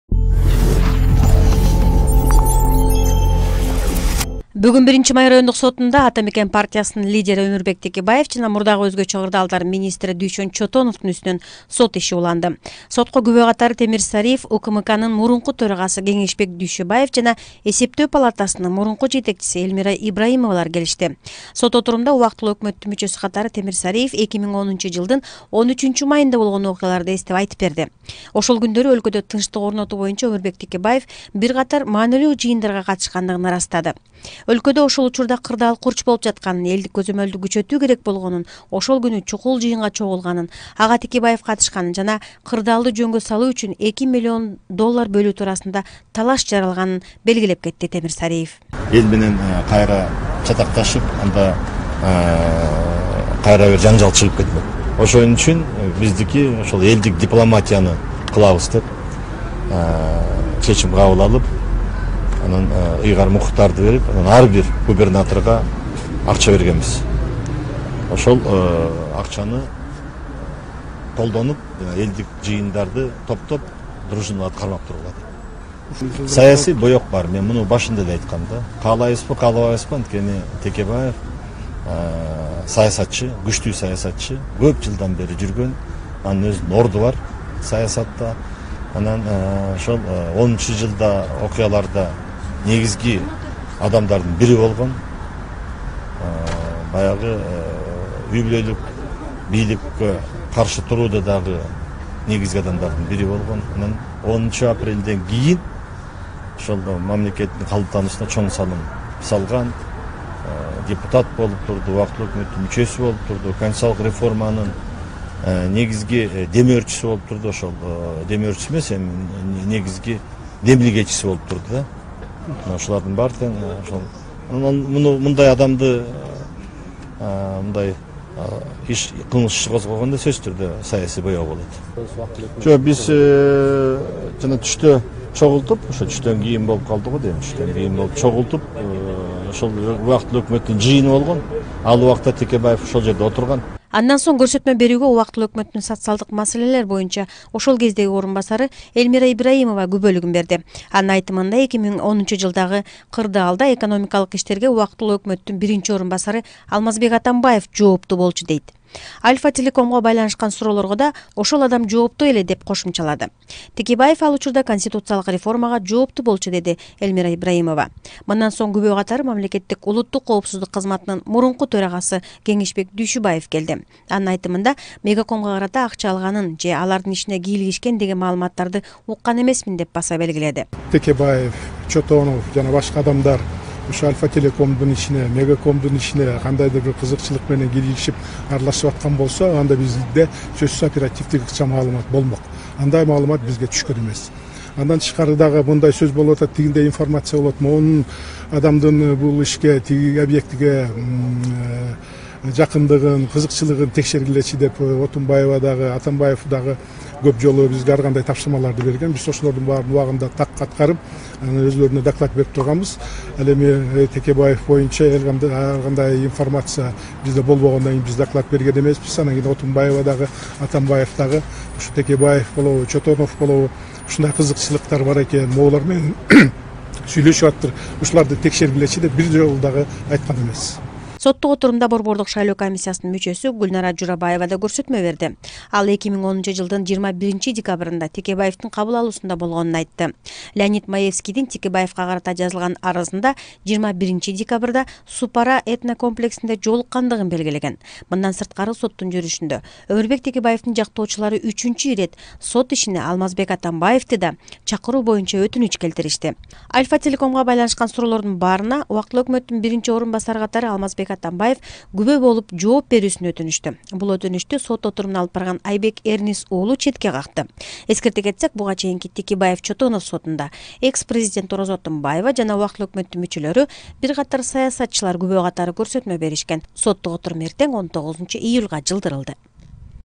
Текебаевтин сотунда Темир Сариев күбө катары катышты
Бугун Биринчи май райондук сотунда Ата Мекен партиясынын лидери Өмурбек Текебаев жана мурдагы өзгөчө кырдаалдар министри Дуйшөн Чотоновдун устунөн сот иши уланды.Сотко күбө катары Темир Сариев УКМКнын мурунку төрагасы Кеңешбек Дүйшөбаев жана Эсептөө палатасынын мурнку жетекчиси Эльмира Ибраимовалар келишти.